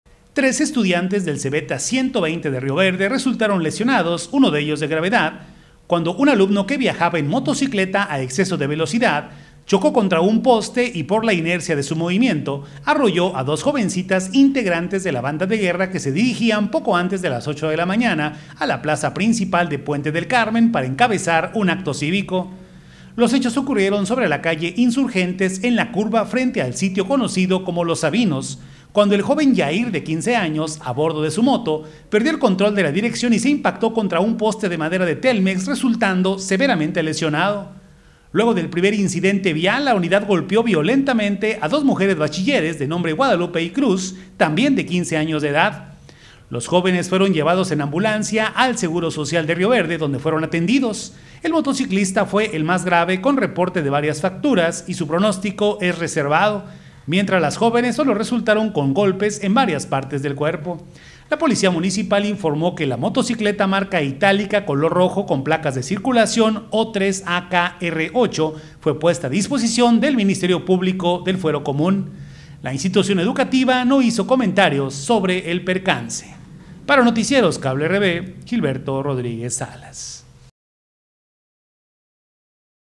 Herido estudiante del CBTA 120 tras chocar en moto y arrollar a dos compañeras de la banda de guerra
Los hechos ocurrieron esta mañana de martes en la calle Insurgentes de Rioverde